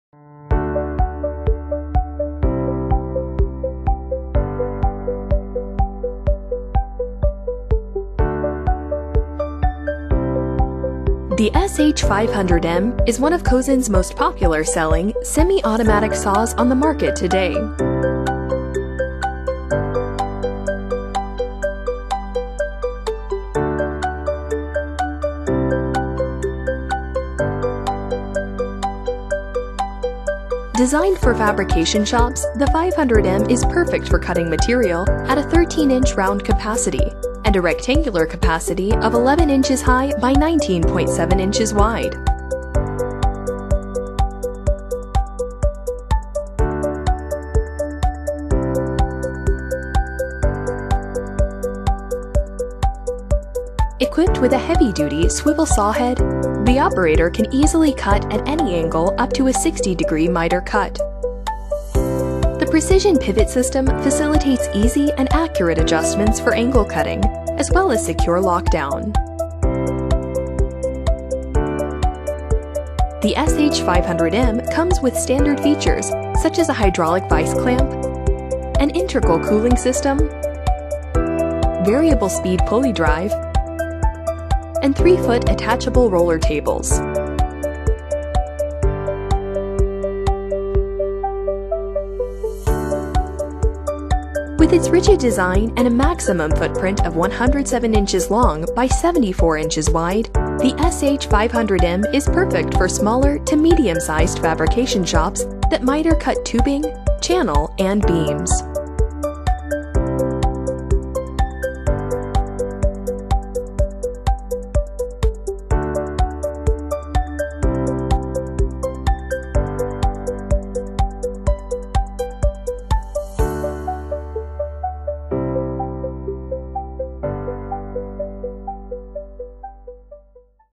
Cosen's SH-500M
A Horizontal Scissor Style Band Saw
Semi-Automatic

Round Capacity: 13"
Rectangular Capacity: 11" x 19.7"

Features:
• Swivel saw head allows miter cutting from 0 degrees to 60 degrees
• Hydraulic cylinder plus precision ground hinge support
• Hardened worm gear for durability
• Hard-stops at, 45, and 60 degrees for quick angle set up
• Blade drive equipped with variable pulley system
• Adjustable flood coolant system
• Motor driven, synchronized wire blade brush
• Carbide blade guides and bearing provides additional blade support
• Rigid cast-iron guide arms
• Cast-iron hydraulic vise
• Adjustable down feed control
• Adjustable material stop
• Saw head automatically raises after each cut